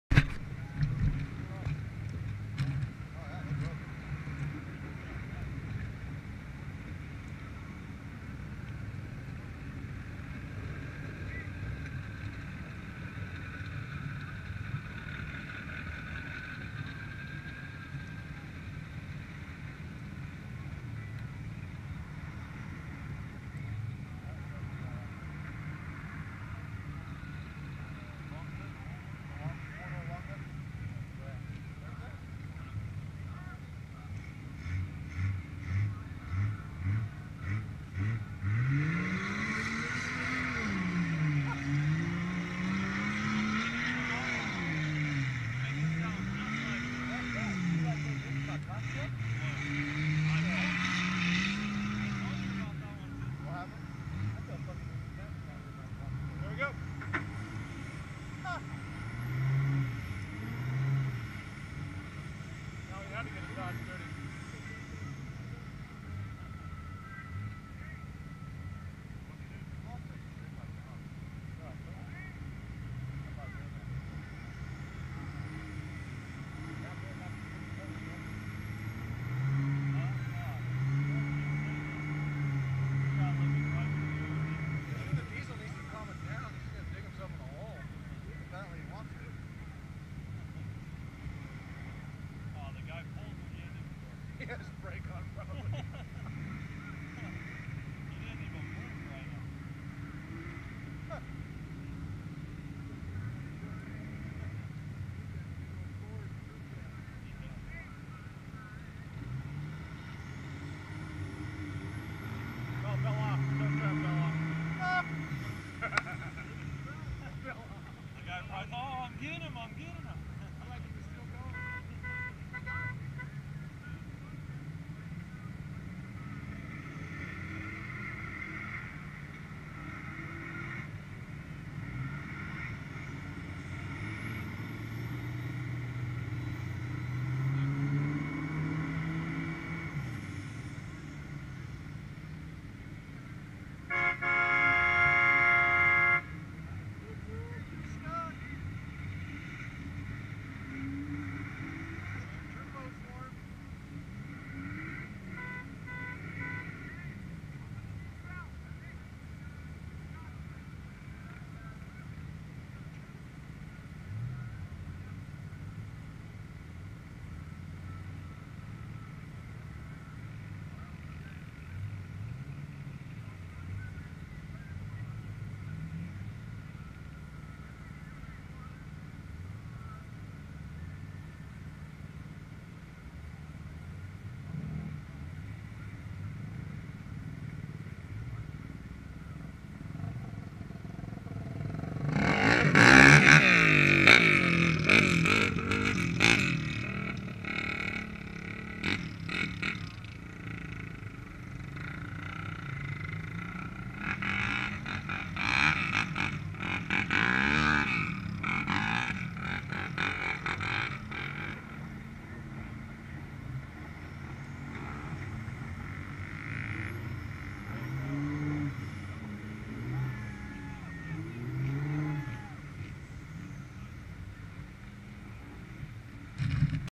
LaGrange Mudfest 11-30-2013 Large Mud Pit Stuck Dodge Diesel Getting Pulled by Gas Chevy
Stuck Dodge Diesel MegaCab getting pulled out by a Chevy at LaGrange Mud Fest